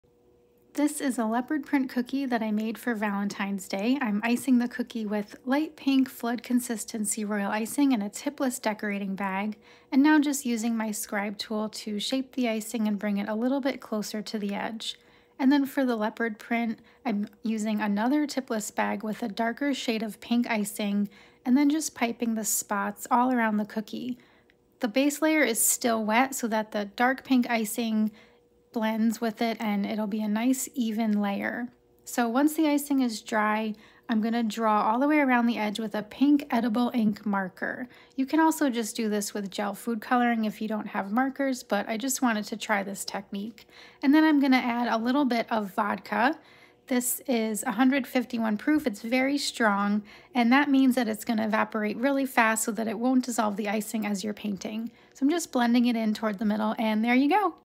How I made a pink leopard print cookie for Valentine’s Day with wet-on-wet royal icing
I made this pink leopard print cookie with wet-on-wet royal icing and an edible ink marker.
All of my recipes and full length step-by-step video tutorials are available in my Cookie Art Club